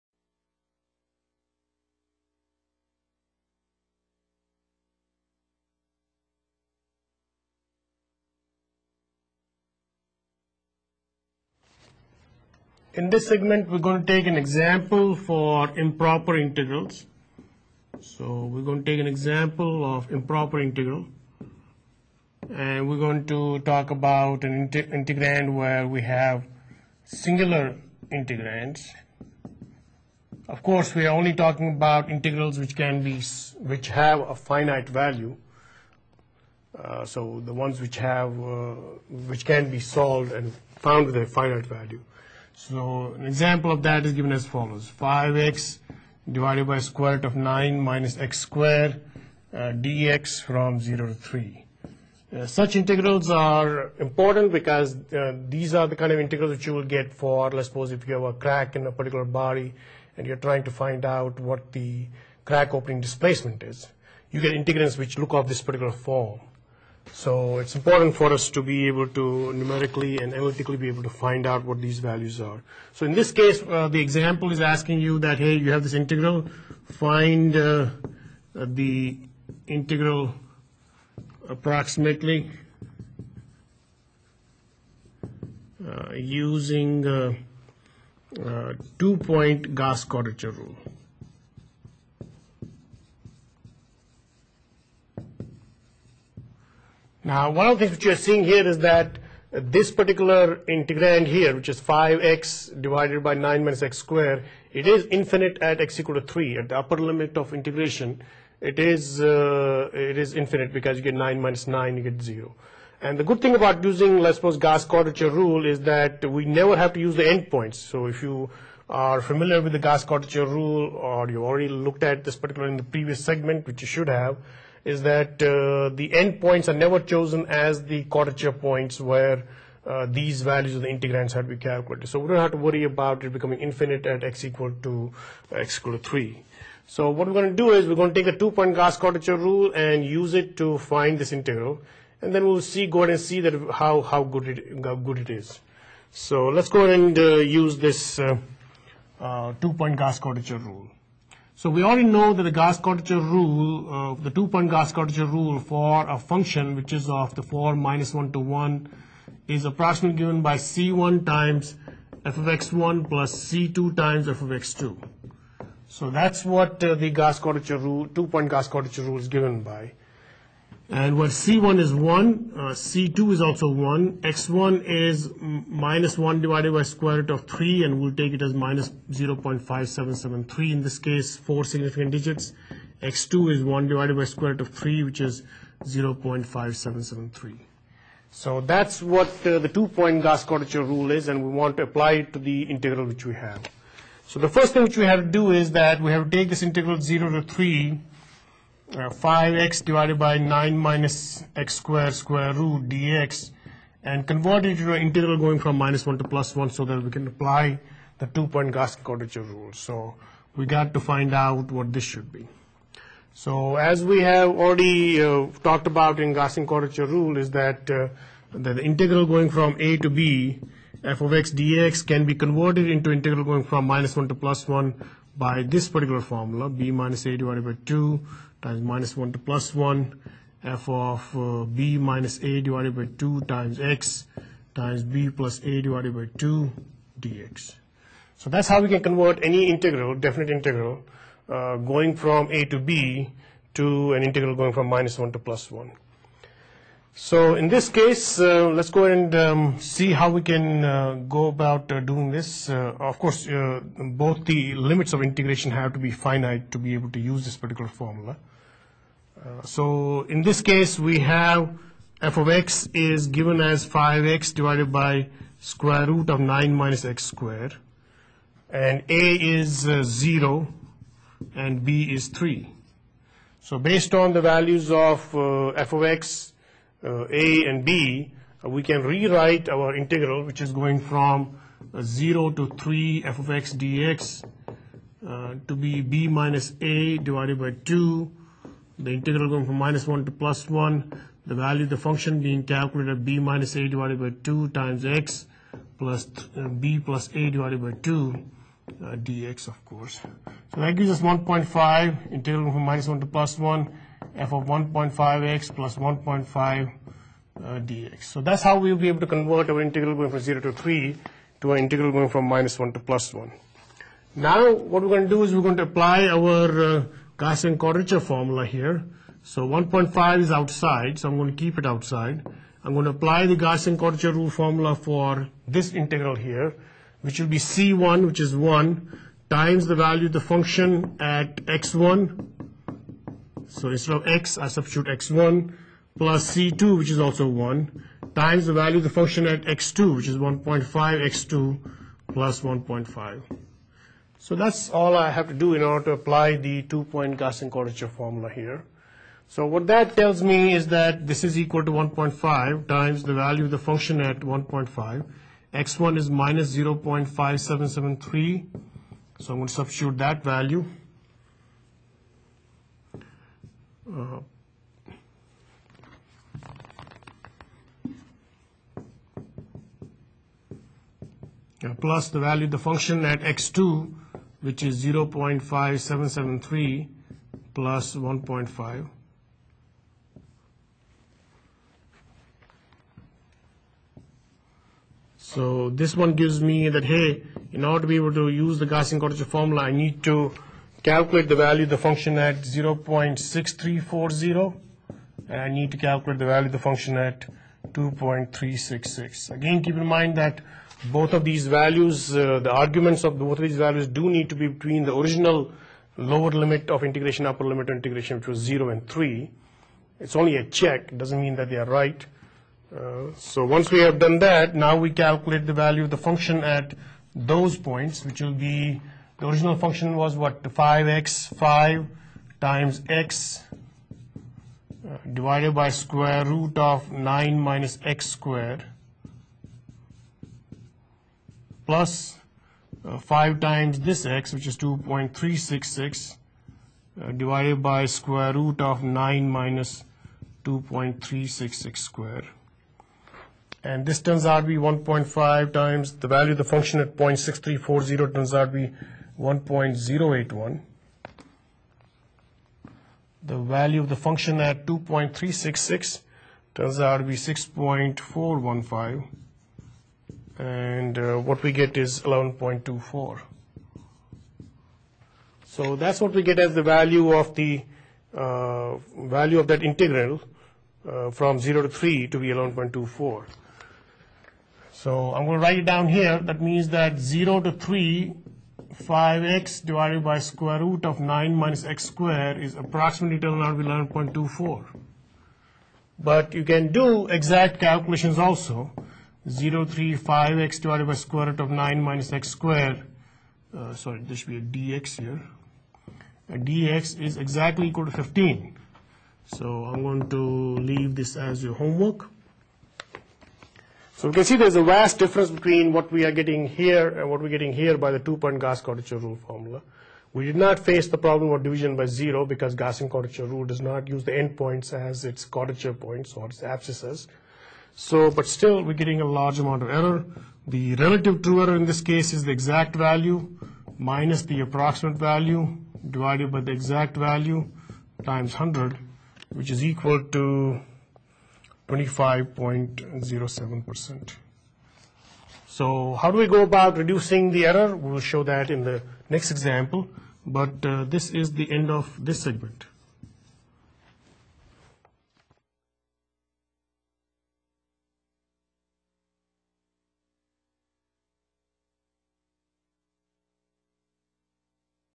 Chapter 07.07: Lesson: Improper Integration: Singular Integrand: Example
Learn via an example how an improper integral with a singular integrand is solved using the Gauss quadrature rule. For more videos and resources on this topic, please